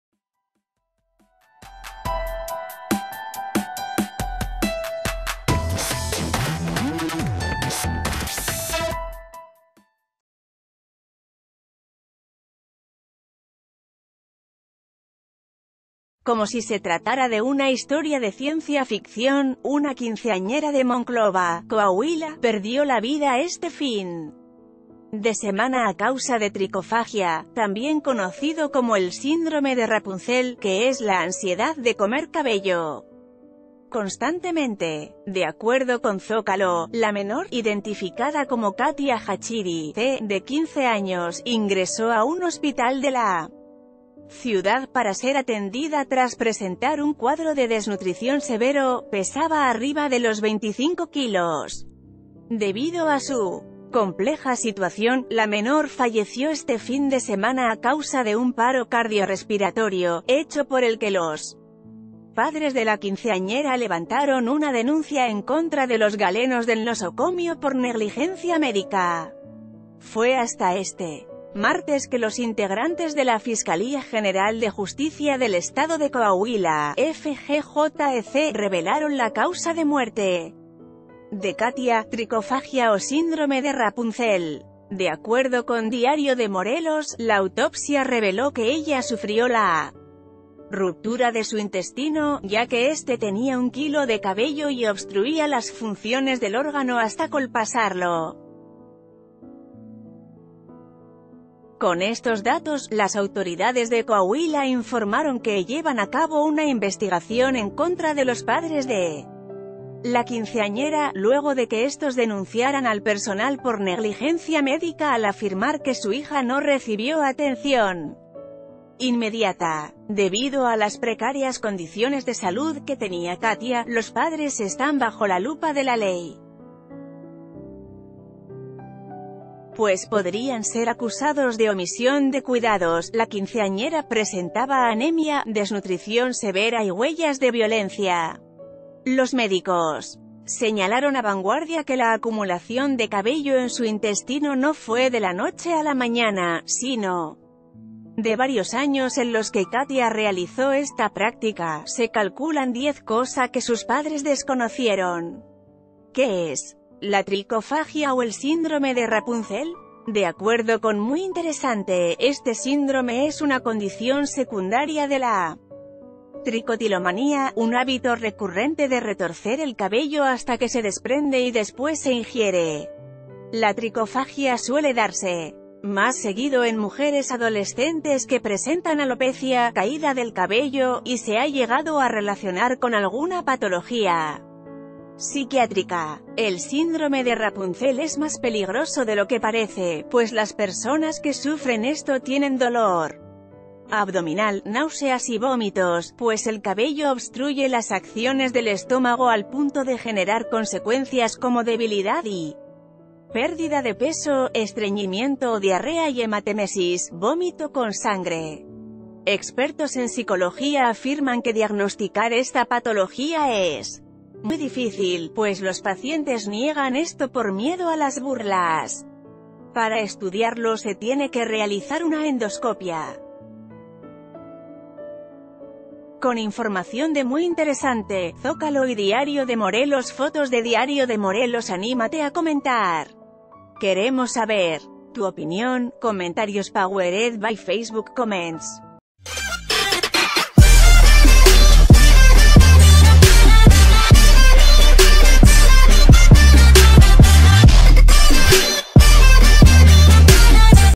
Estremece atroz muerte de quinceañera con tricofagia en Coahuila
Como si se tratara de una historia de ciencia ficción, una quinceañera de Monclova, Coahuila, perdió la vida este fin de semana a causa de tricofagia, también conocido como el síndrome de Rapunzel, que es la ansiedad de comer cabello constantemente. De acuerdo con Zócalo, la menor, identificada como...